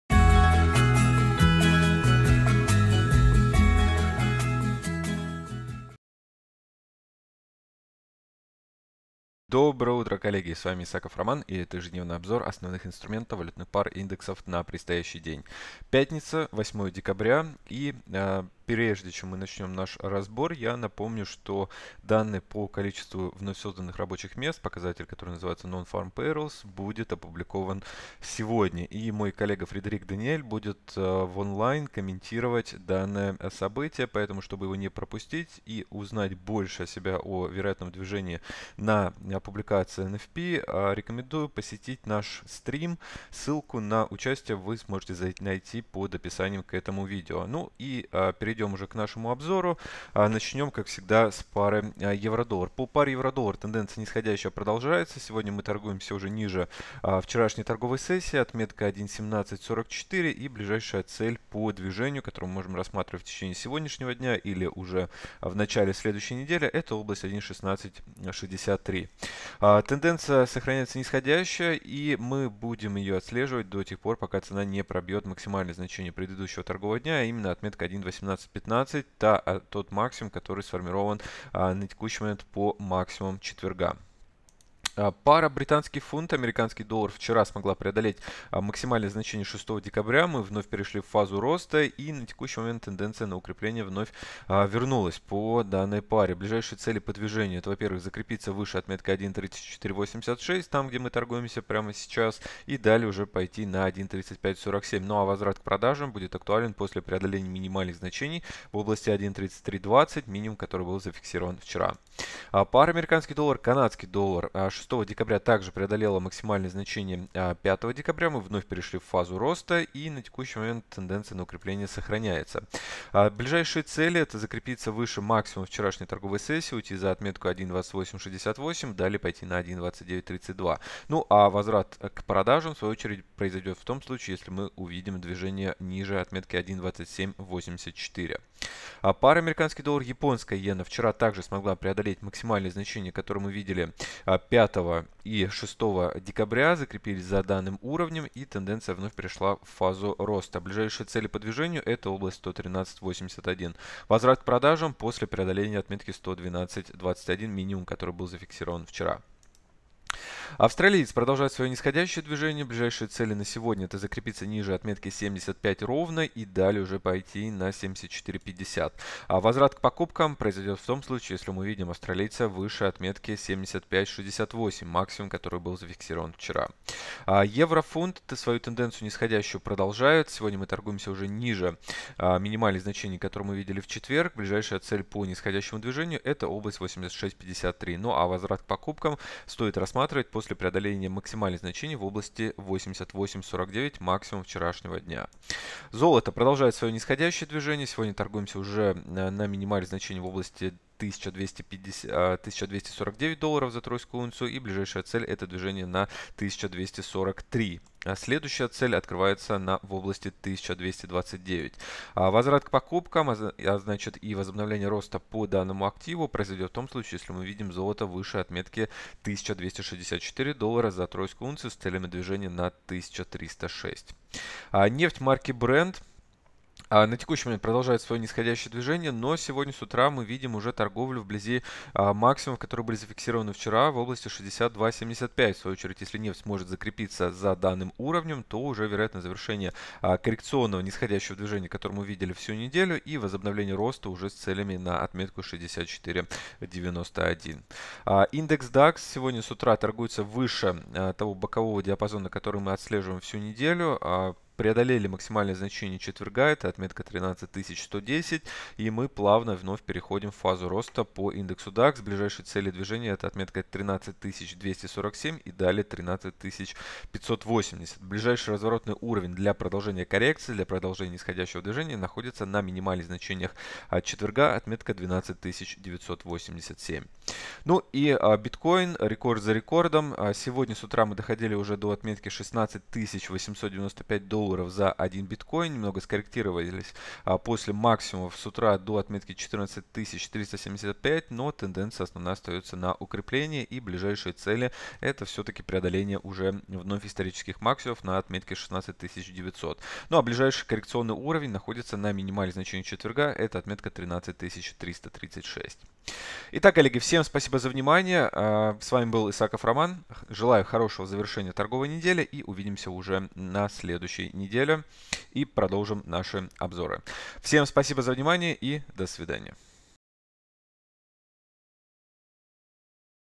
Прогноз рынка форекс на основе системы Price Action с Романом Исаковым 8 декабря 2017
Еженедельный авторский вебинар, посвященный анализу текущей ситуации на рынке